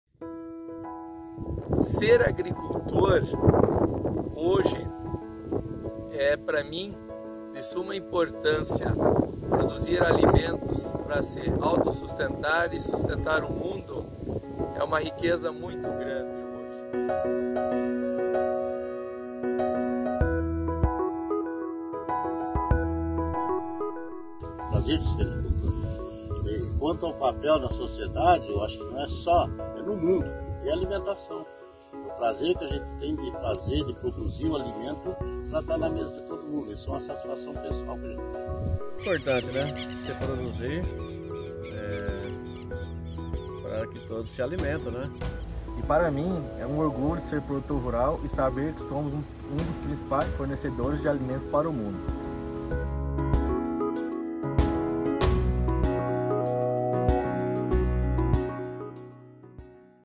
Dia do Agricultor: A agricultura brasileira é feita de muitas histórias
A Agricultura brasileira é feita de muitas histórias. E ouvir os protagonistas destas histórias contando suas experiências nos enche de orgulho e admiração pelos agricultores do nosso país.

Para celebrar o Dia do Agricultor, que acontece neste dia 28 de Julho, compartilhamos ao longo dos últimos dias algumas dessas histórias como forma de homenagear todos os homens e mulheres que vivem essa linda profissão.